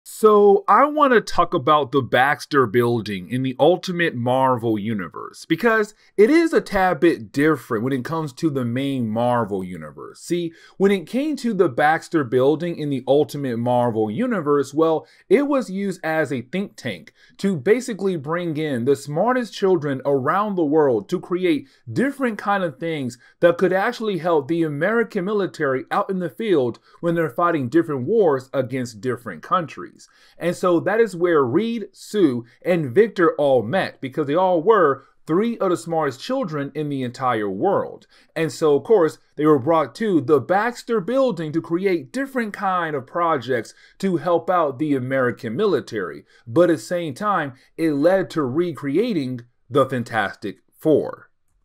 The Ultimate Baxter Building Think Tank
Hey there YouTube, welcome back to another Ultimate Marvel Moment, where we pick up with the Baxter Building being a Think Tank in the Ultimate Marvel Universe.